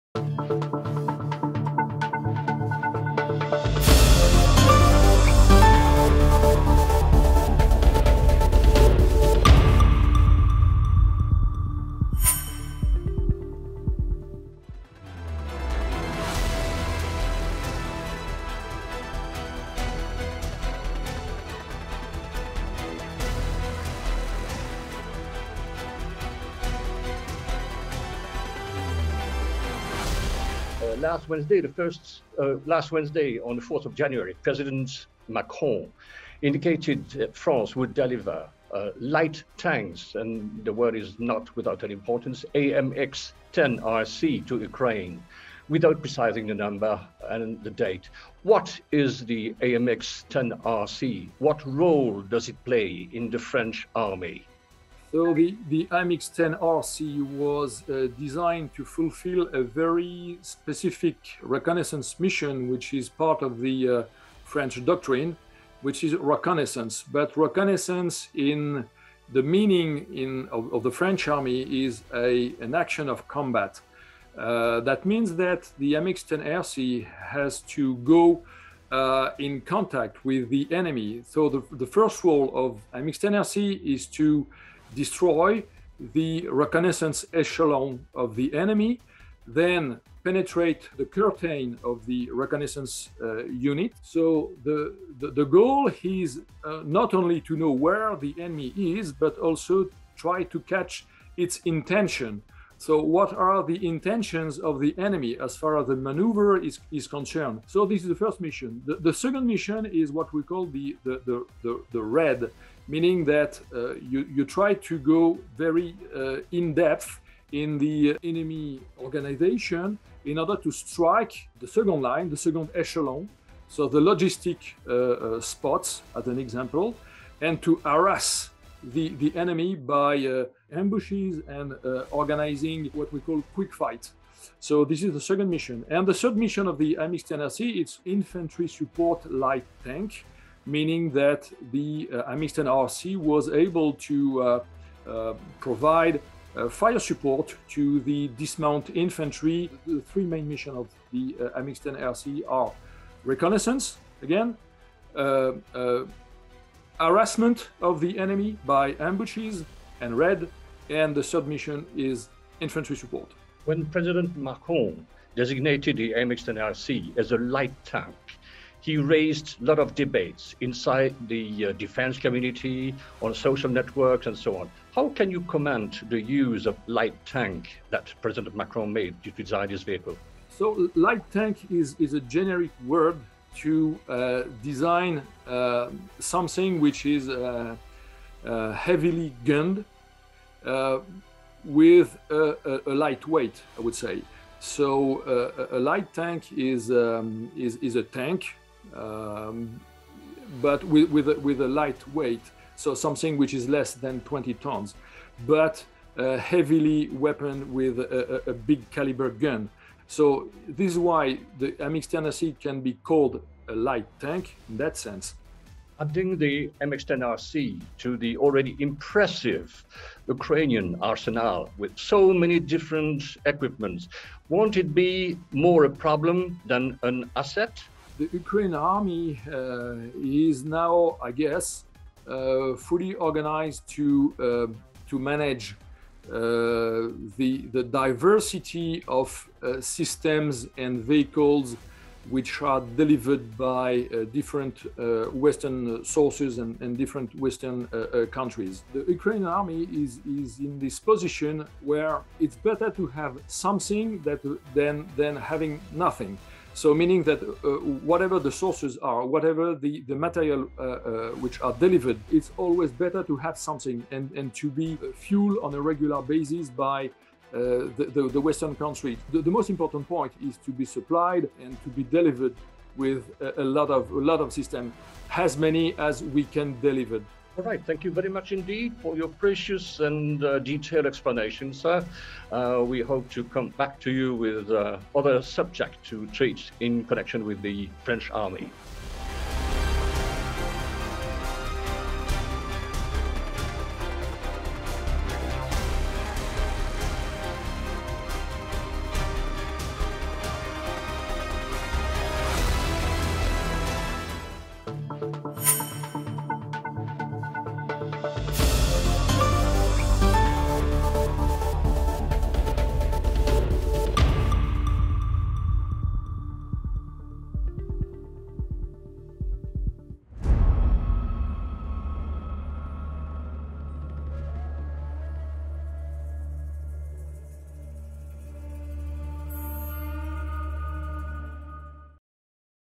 Discover & know everything French AMX-10RC anti-tank vehicle combat capabilities for Ukraine
Exclusive video interview about the French AMX-10RC with Marc CHASSILLAN, a French military expert on armored vehicles. According to a statement published by the French DoD , the French Ministry of Defense Sébastien Lecornu agreed on the delivery of the first batch of anti-tank reconnaissance armored vehicles AMX-10RC to the Ukrainian army that would take place within two months.